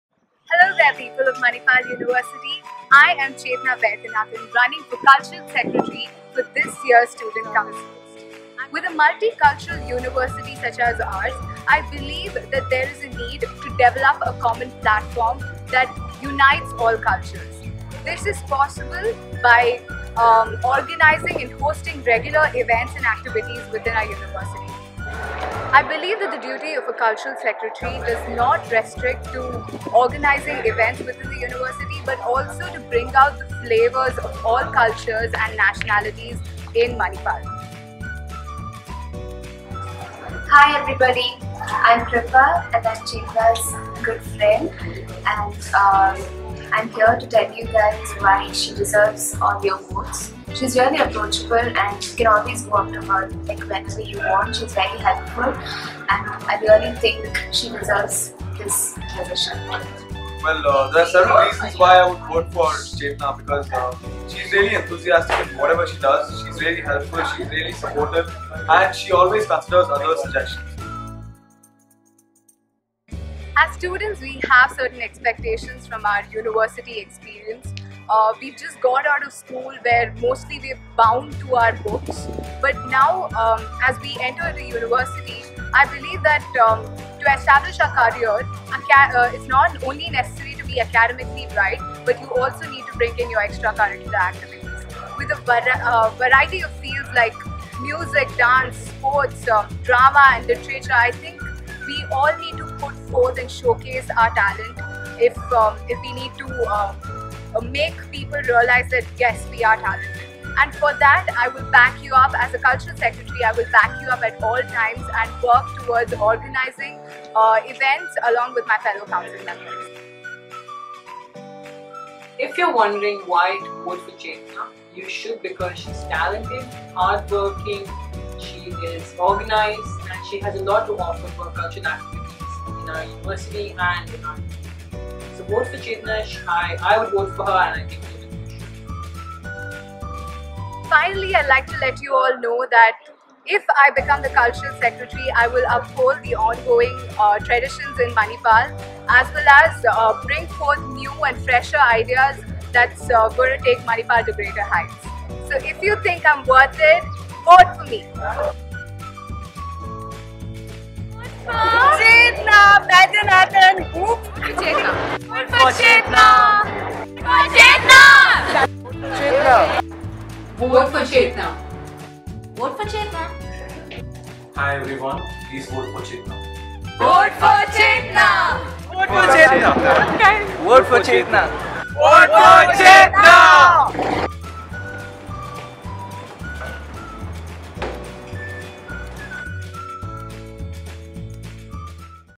MUD Student Council Campaign Video
Hi, I am Chetna Vaidyanathan, running for Cultural Secretary for the upcoming student council elections at Manipal University Dubai.
Check out my video, if you want to know why you should be voting for me! :)

I'd like to thank everyone who was a part of this video and supported me throughout this campaign. Special thanks to my professors who didn't shy away from featuring in this video. You are the best faculty any student could ever have.
Also, three cheers to Mehak Srivastava for being so patient while shooting, and Mokshada Amur for snapping my picture for the campaign poster. And thanks to all those friends who made this an incredible experience.

Loads of love for my parents and sister for helping me out as usual :D